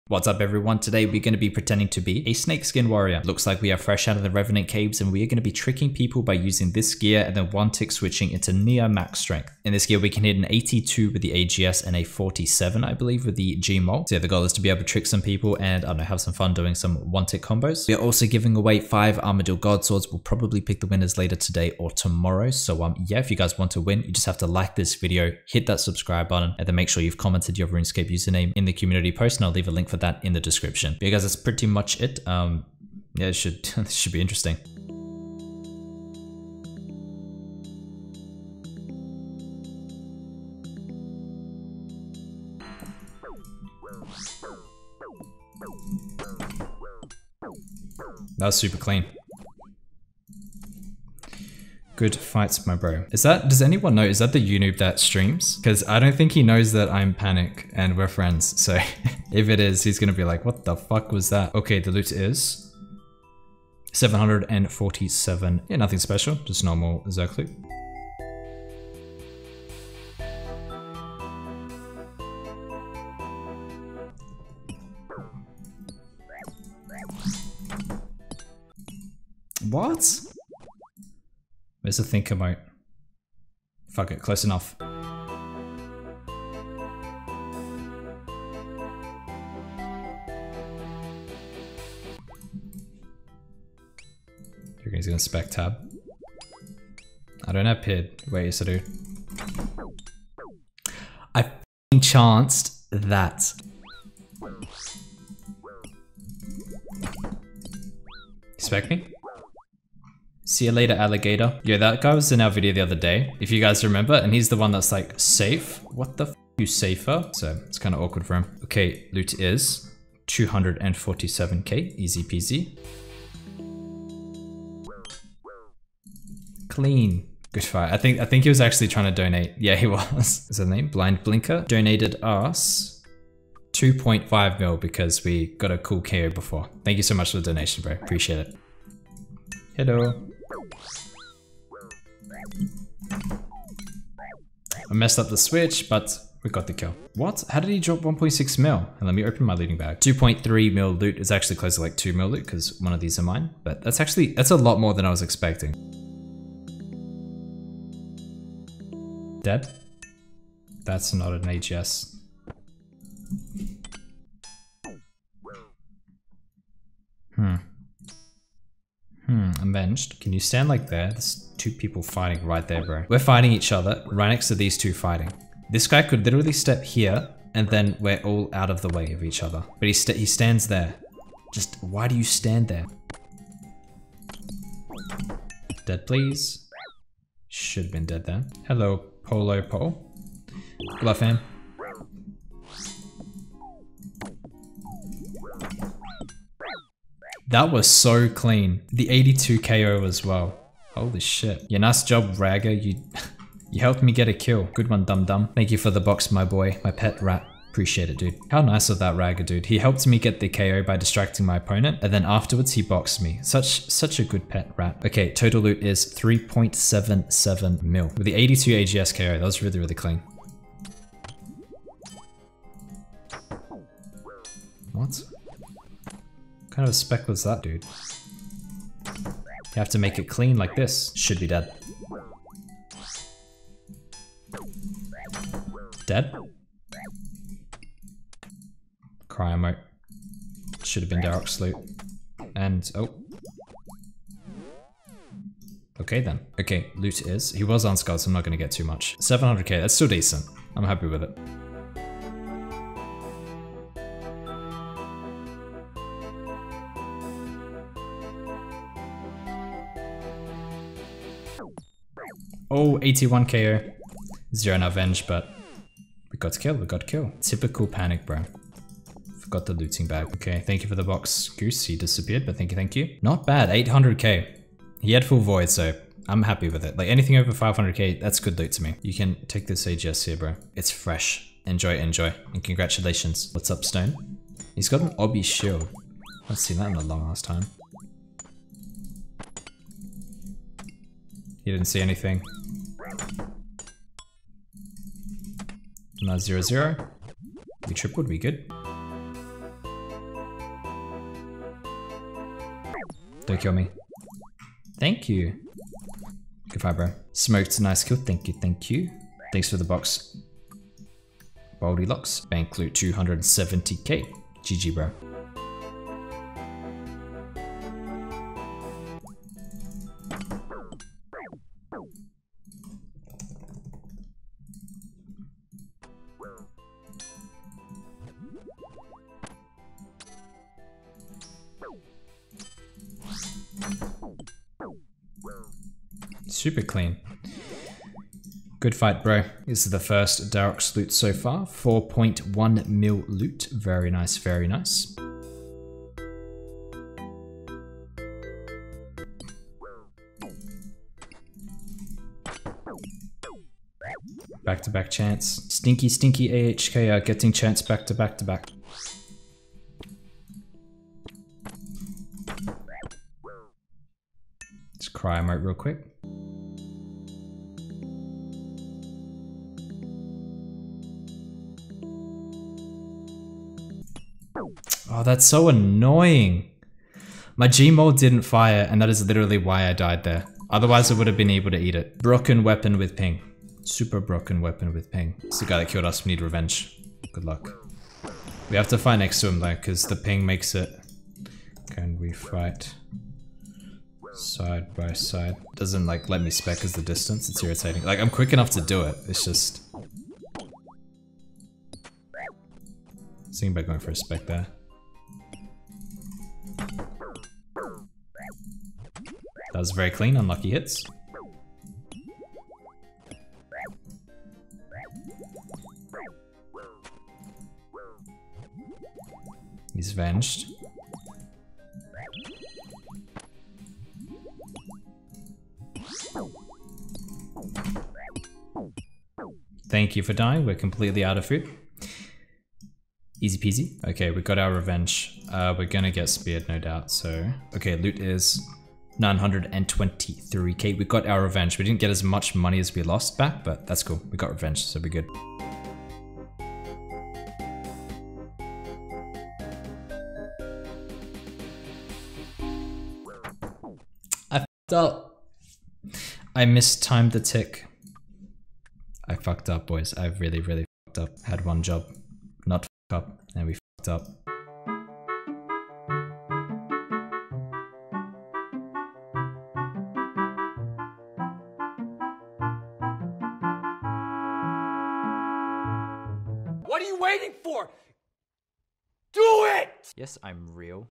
Pretending to be a Noob (Little do they know, I have a 6-way Switch)
We are in full snakeskin gear, looking like a Rev Cave bot! The goal is to try and bait pkers into thinking I'm a noob, and then we pull out our 1-tick max strength combo to try and get the PK.

► CC: The Pure Seb